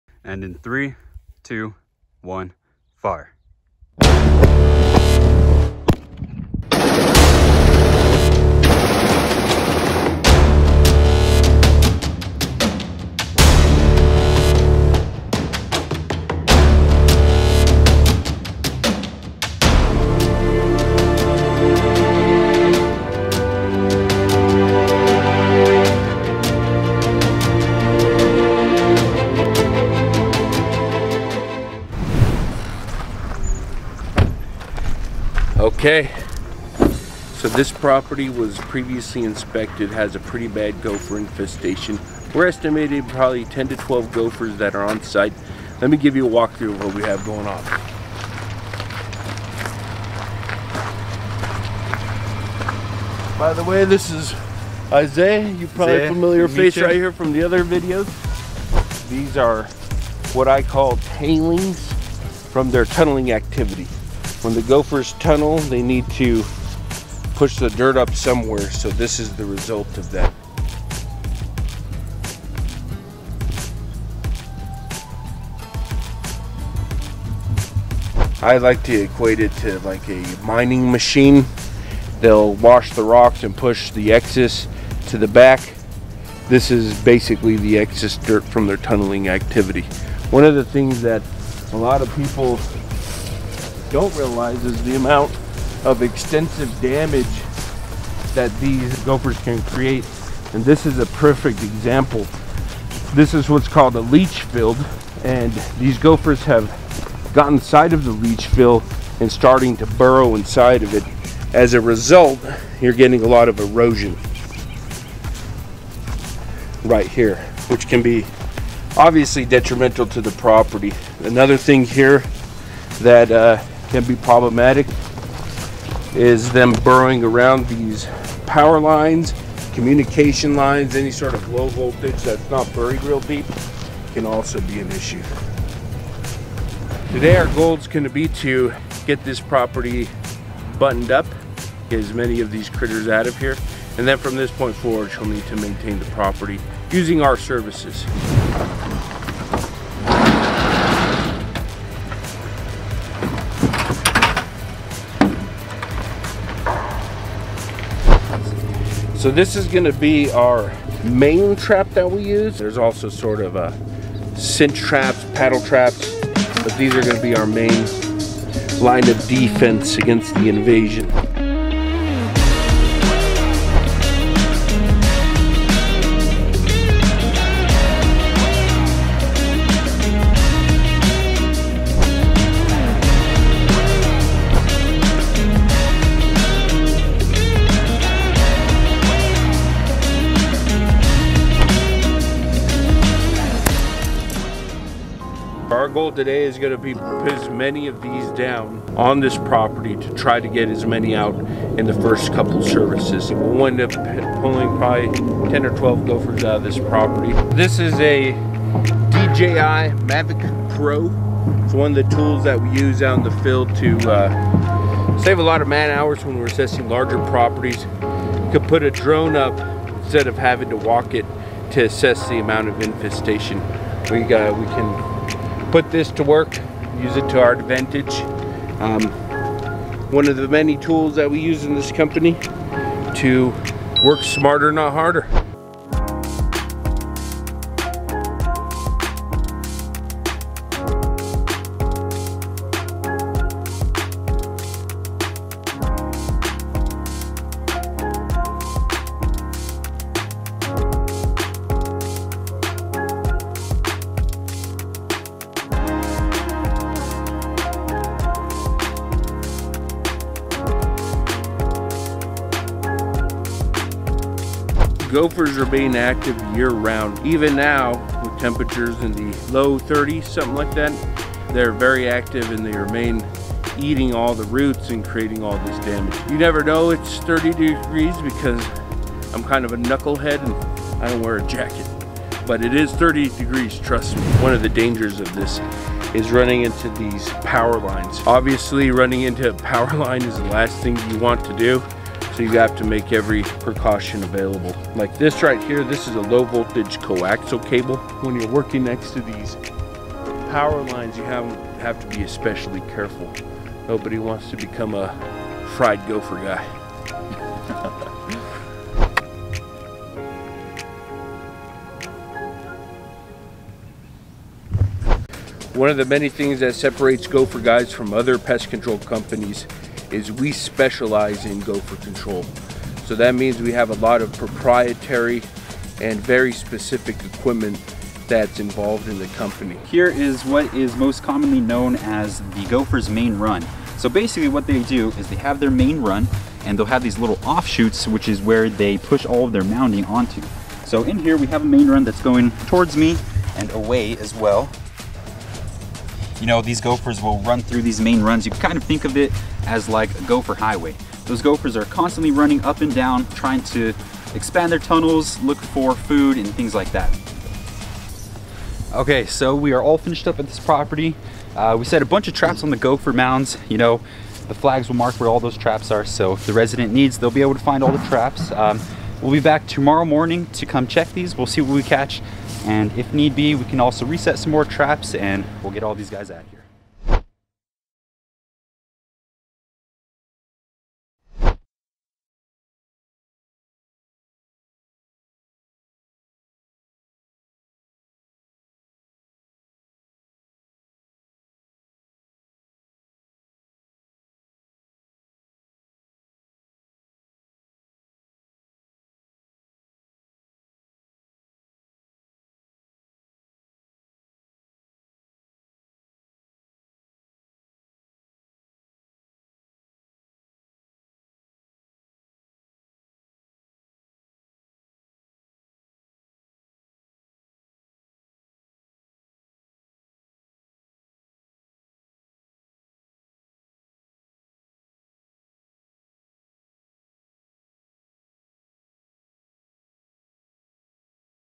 Operation Gopher: Gopher Damage
Welcome back to Operation Gopher! Today we find lots of gopher damage and showcase the different areas they can get into.

In this series you will find us adventuring and hunting unwanted varmints. From rattle snakes to badgers, we find them all!